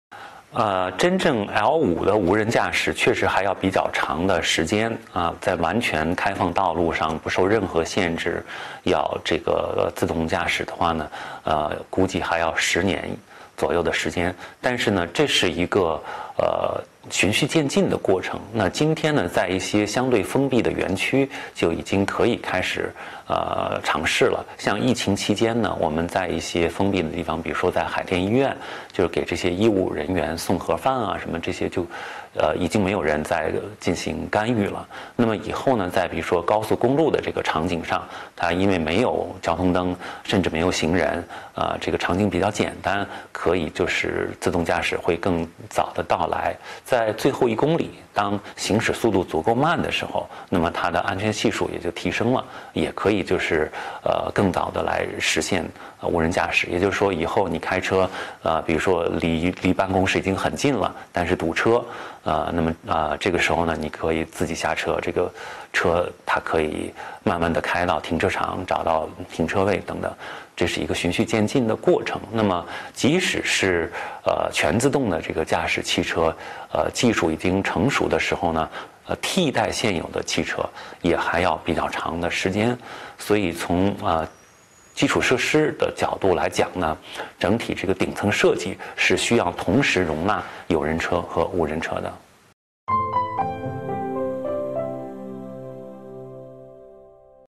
L5自动驾驶何时来到？李彦宏：再等十年
5月21日，全国政协委员、百度董事长李彦宏在接受中新社记者采访时表示，L5级别的自动驾驶估计还要十年左右的时间才能实现。